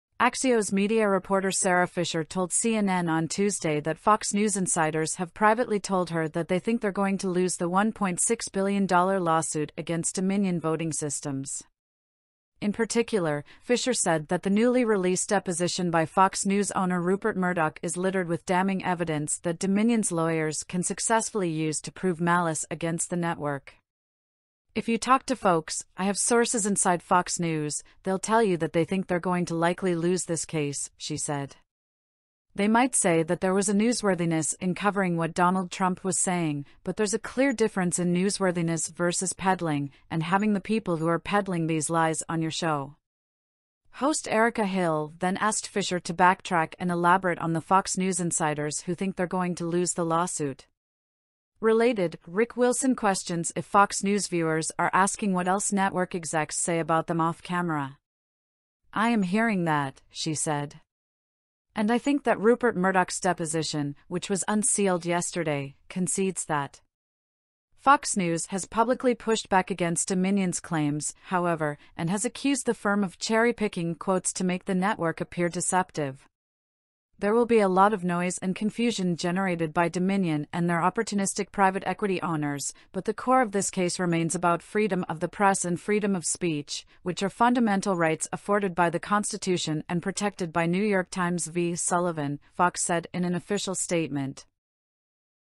Fox News insiders think 'they're going to lose'
Axios media reporter Sara Fischer told CNN on Tuesday that Fox News insiders have privately told her that they think they're going to lose the $1.6 billion lawsuit against Dominion Voting Systems.
In particular, Fischer said that the newly released deposition by Fox News owner Rupert Murdoch is littered with damning evidence that Dominion's lawyers can successfully use to prove malice against the network.